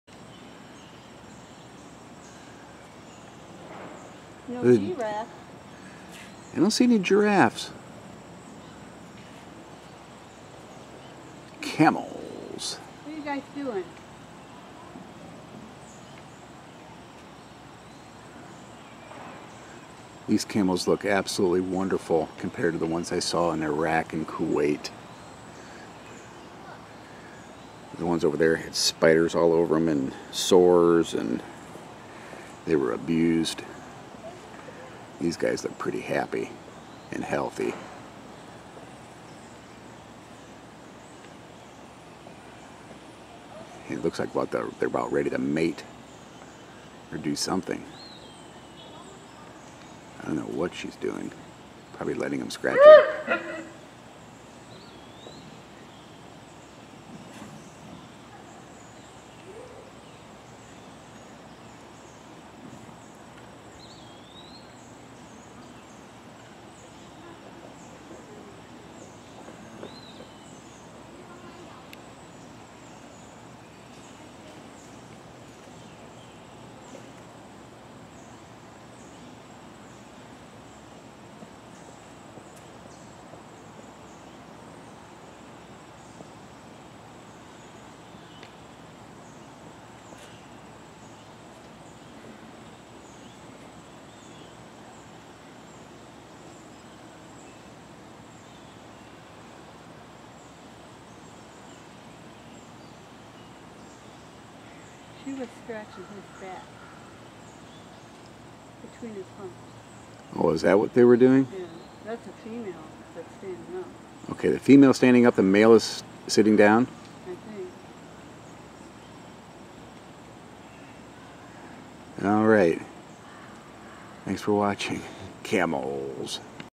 Camels loving each other and one making noise in HD
These are two camels at Potter's Park Zoo in Lansing Michigan. These camels decided to love on each other and make some funny sounds.